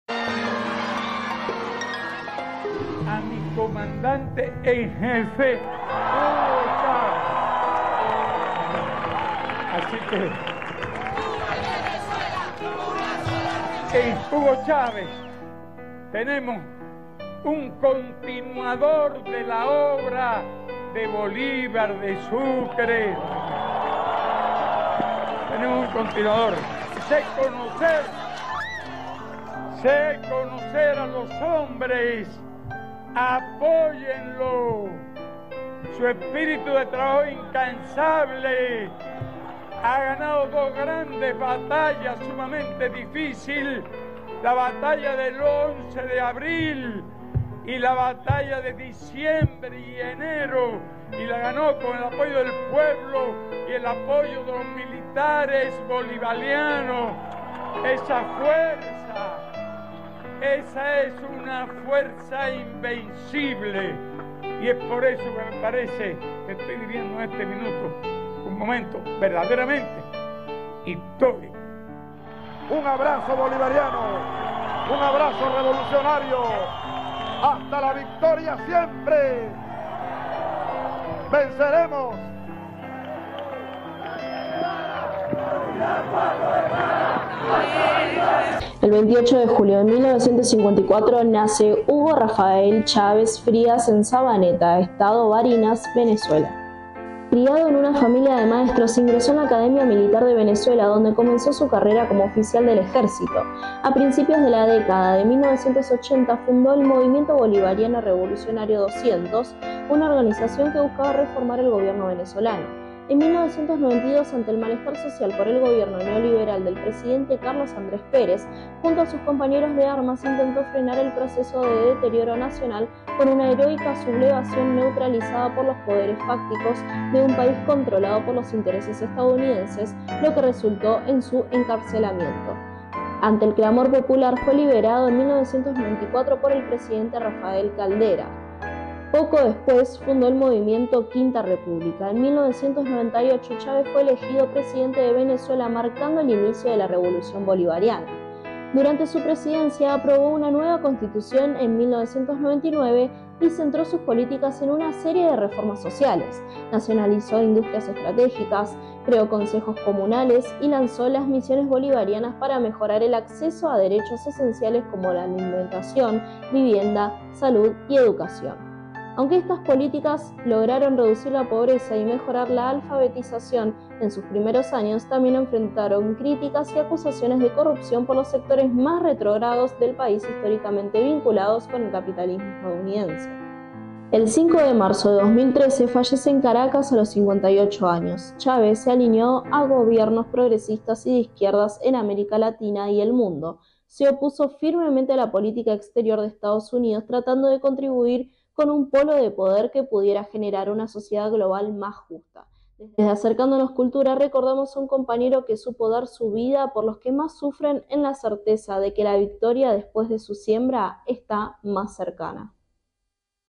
Nacimiento de Hugo Chavez
28 de julio de 1954: Nace Hugo Rafael Chávez Frías en Sabaneta, estado Barinas, Venezuela. Criado en una familia de maestros, ingresó en la Academia Militar de Venezuela y fundó el Movimiento Bolivariano Revolucionario 200 (MBR-200) en la década de 1980 para reformar el gobierno. Tras un fallido golpe de Estado en 1992 y su posterior encarcelamiento, fue liberado en 1994 y fundó el Movimiento Quinta República. En 1998, fue elegido presidente, iniciando la "Revolución Bolivariana" con reformas sociales significativas, incluyendo la nacionalización de industrias y la creación de Misiones Bolivarianas. Chávez se alineó con gobiernos progresistas de América Latina y se opuso a la política exterior de Estados Unidos. Falleció en Caracas el 5 de marzo de 2013 a los 58 años.